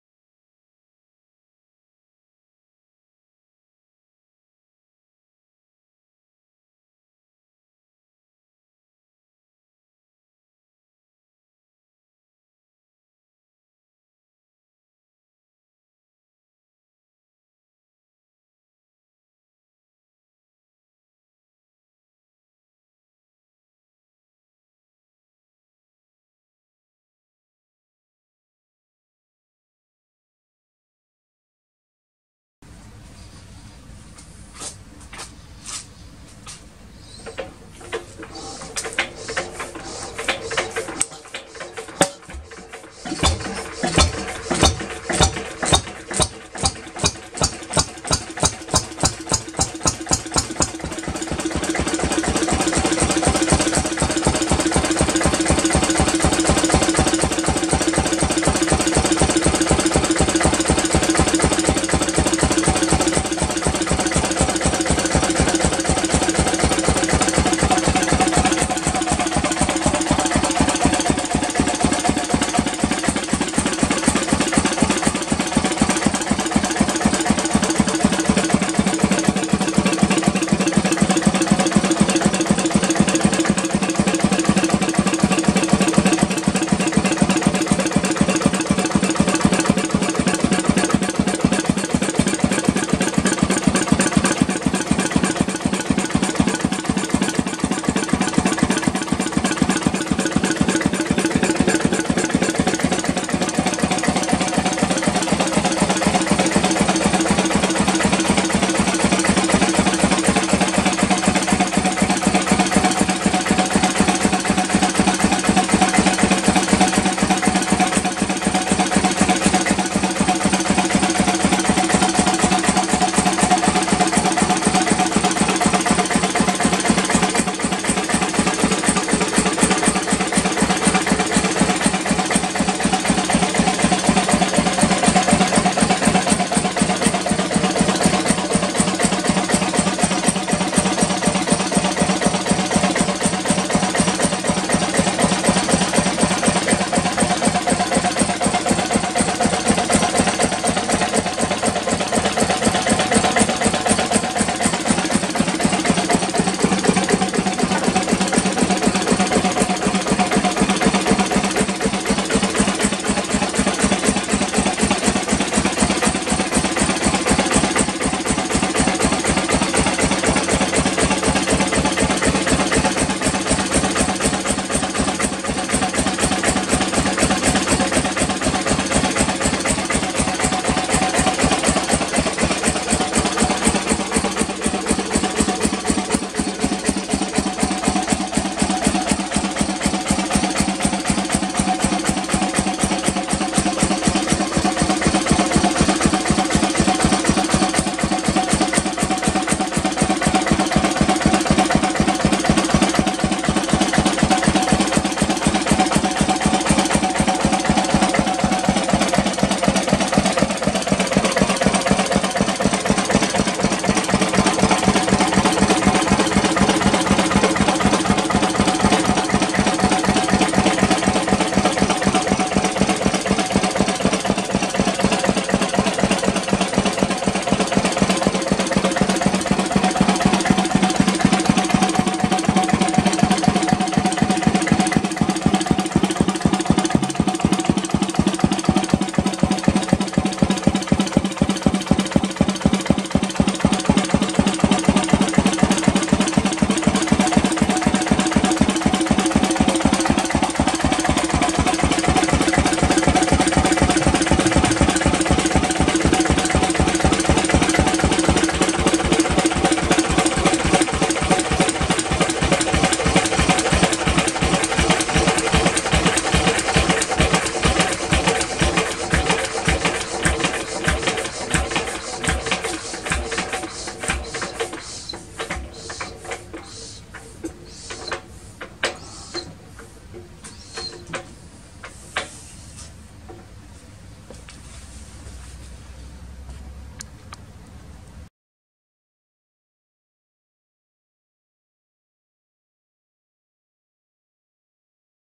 Lister CS 3.5hp short run
LISTER CS 3.5hp at 650rpm.  Made in Dursley, England in 1937.
Single cylinder 'Cold Start' diesel stationary engine.

I acquired this engine in 2009 and during 2017 and 2018, the engine was completely overhauled by me from bottom to top after having stood idle in a farm shed for about 30 or 40 years.  The farm was near Waroona, Western Australia and the engine was originally supplied by 'Malloch Bros' Perth, Western Australia.
It is believed to have originally powered a small lighting plant on the farm.  When electricity was finally laid to the farm probably in the late 1940s, the engine was re-purposed to drive milking machinery out in the dairy, most likely driving a vacuum pump.

For this shorter video, I have recently fitted a smaller capacity cooling tank and re-mounted the fuel tank more rigidly. Also reinforced the underside of the trolley with 2 lengths of heavy duty angle iron to reduce 'bend' in the trolley due to torque pulses of the engine on the power stroke.